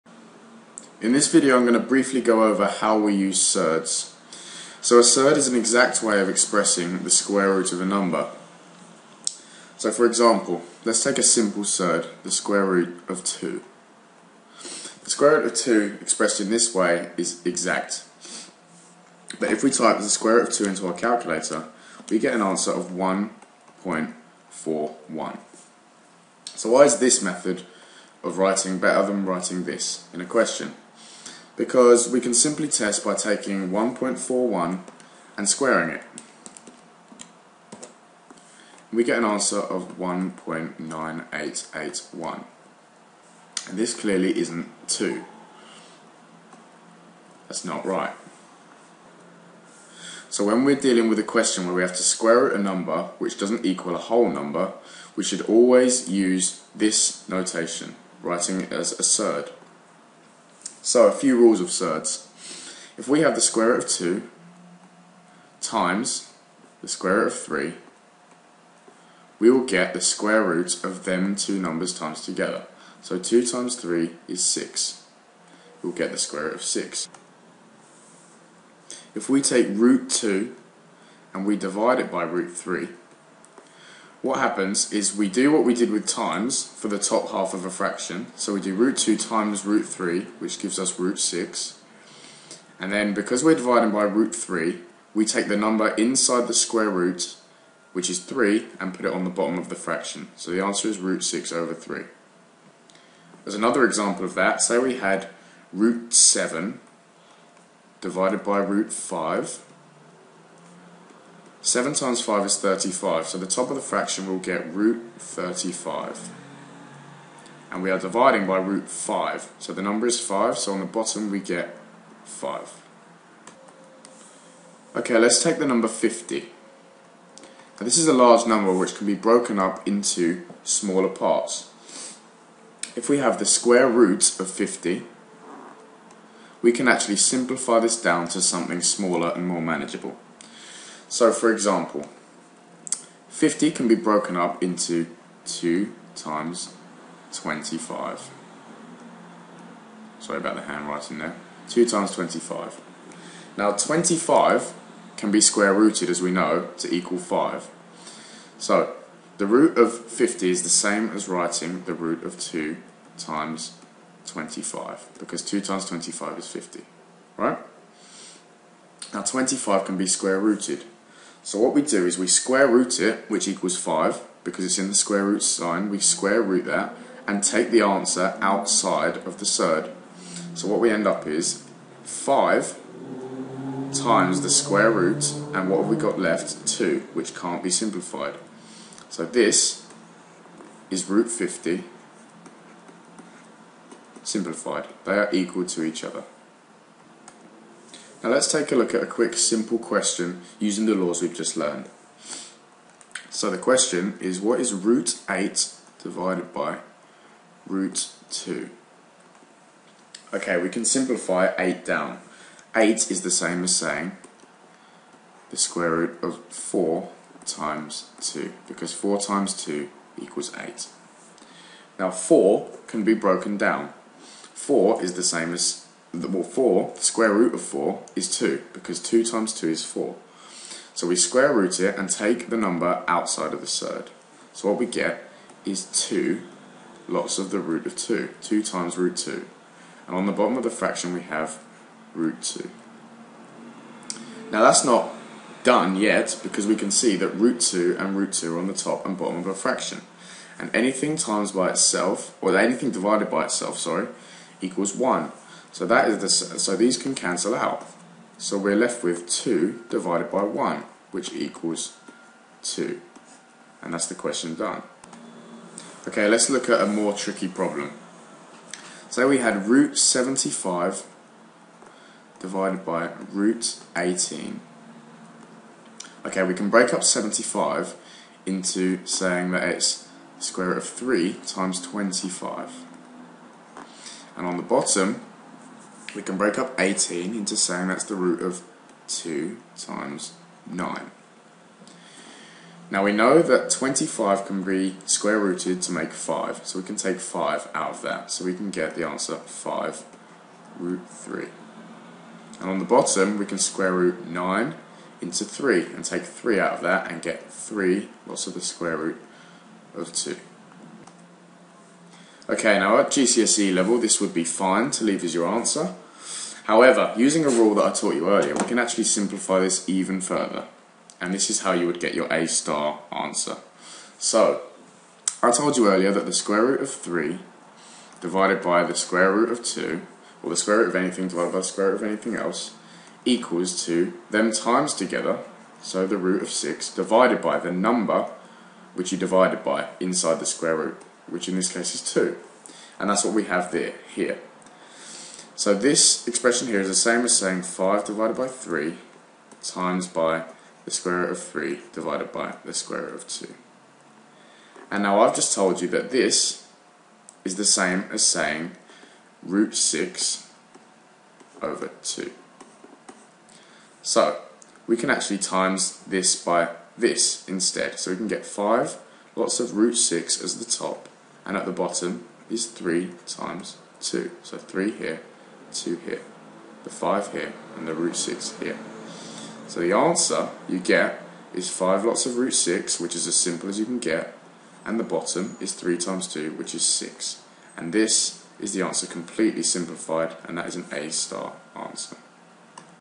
GCSE Maths Surds (Laws of Surds)
In this video I explain the laws of surds, and how surds are useful in calculations. They are the exact form of the square root of a number